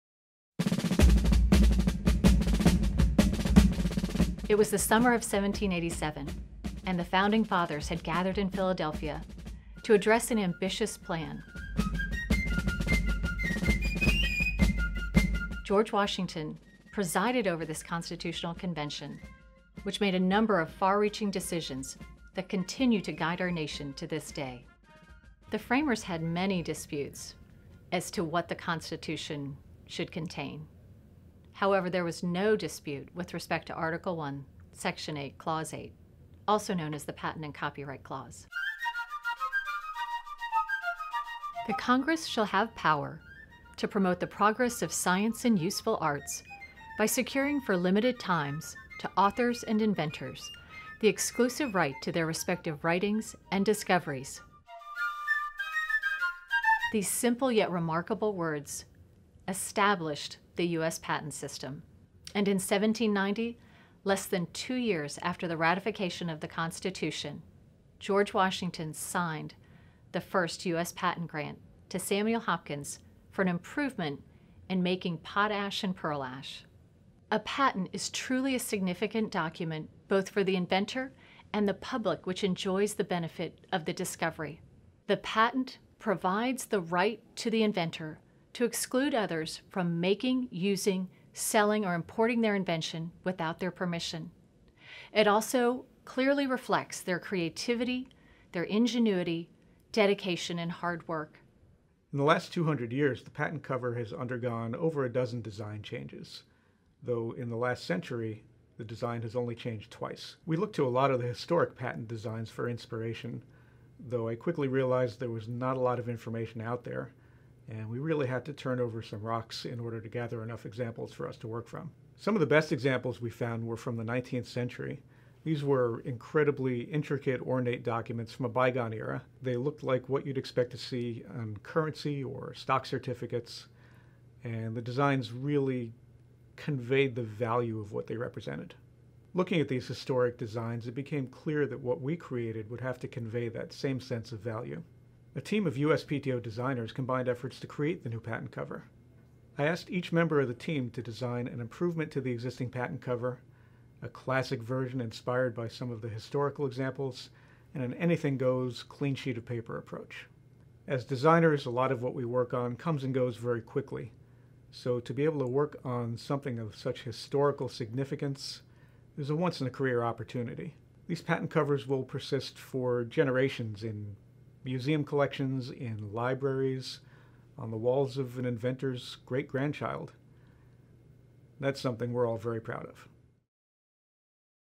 10 million patents
The USPTO has created a new patent design in advance of issuing the 10 millionth patent in the summer of 2018. Step back to 1790 when the first patent was signed by President George Washington, and discover the historical significance of patents and patent covers designs through the years.